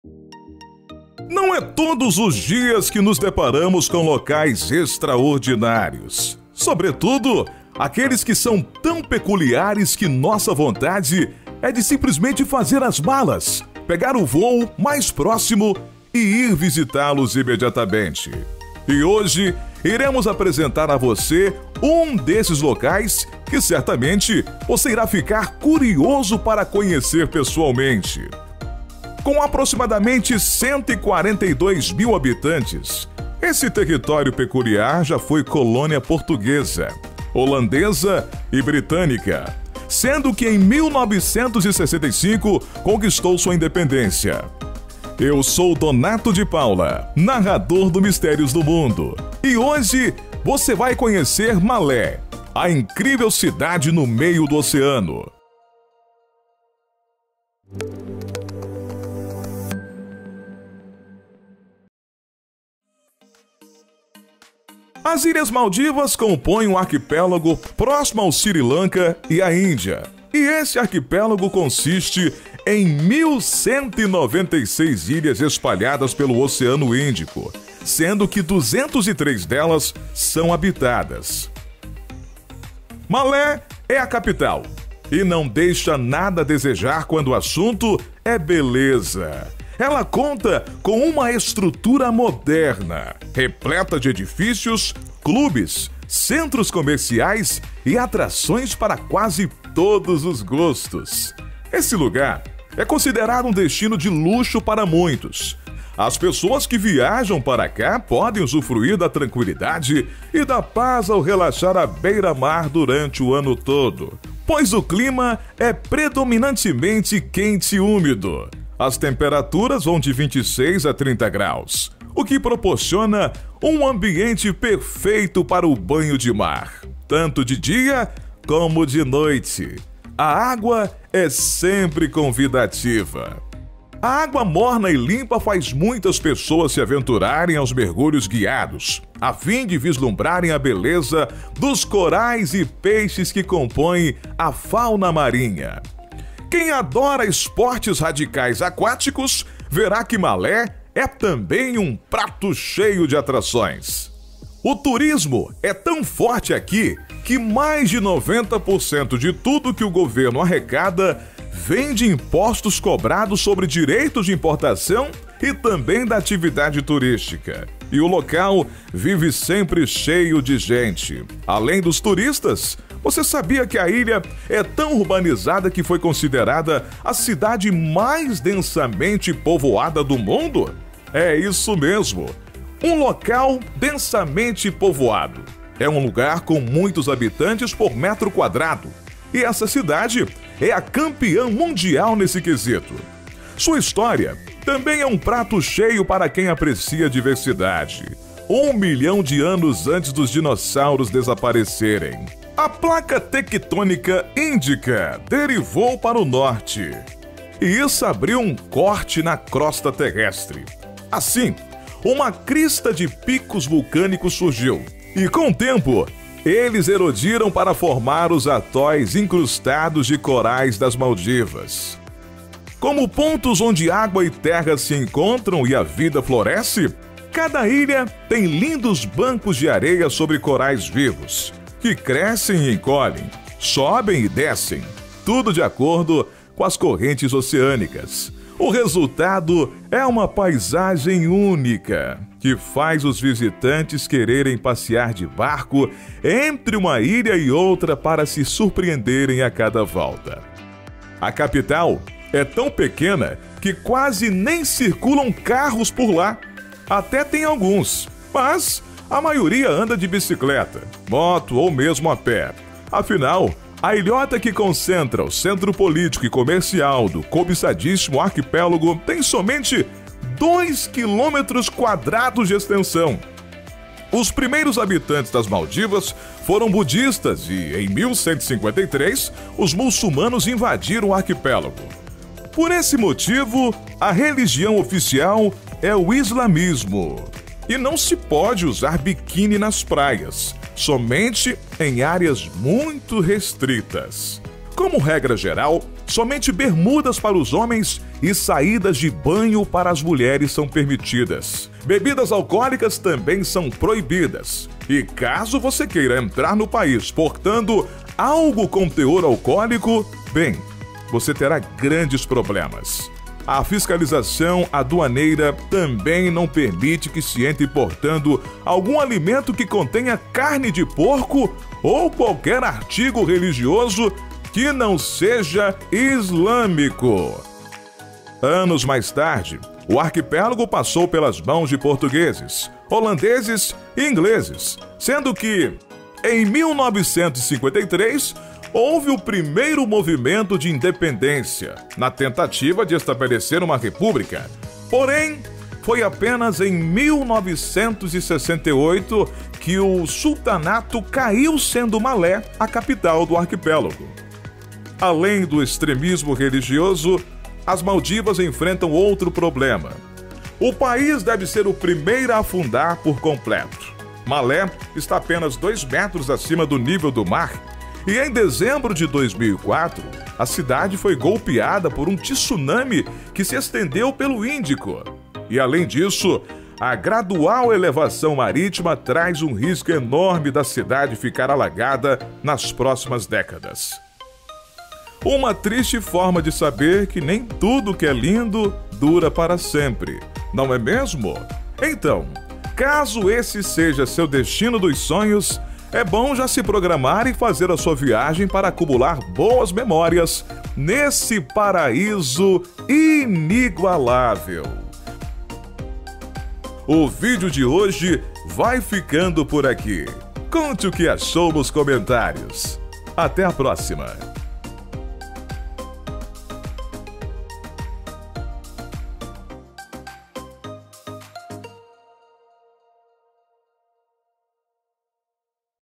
Malé, a incrível cidade no meio do oceano
Malé, a excêntrica capital das Maldivas, é uma das cidades mais densamente povoadas do mundo. Conheça mais sobre esse lugar peculiar em nosso vídeo.